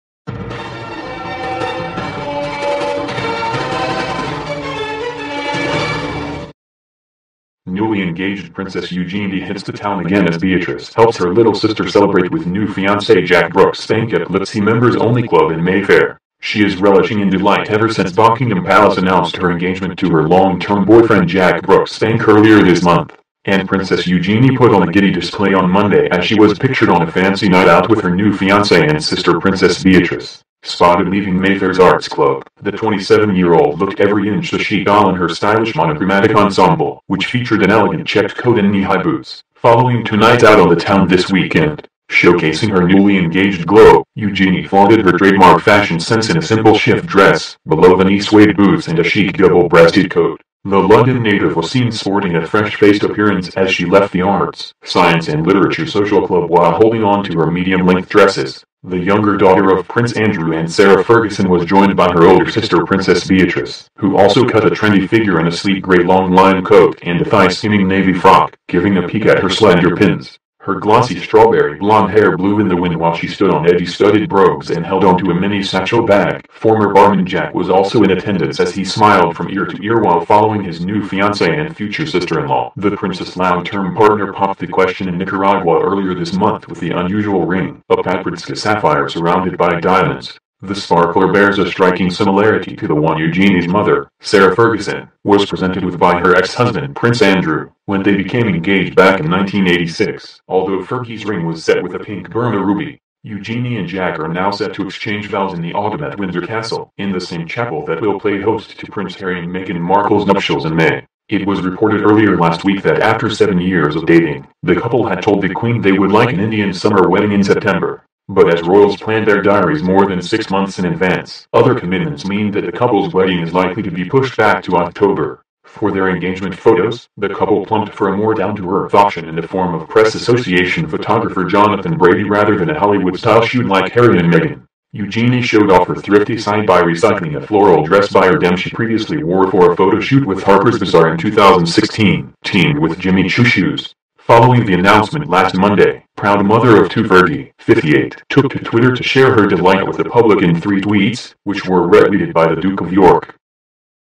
Princess Eugenie enjoys a night out with fiancé and sister he in Mayfair
Newly-engaged Princess Eugenie hits the town AGAIN as Beatrice helps her little sister celebrate with new fiancé Jack Brooksbank at glitzy members-only club ... . Video Title Princess Eugenie enjoys a night out with fiancé and sister he in Mayfair .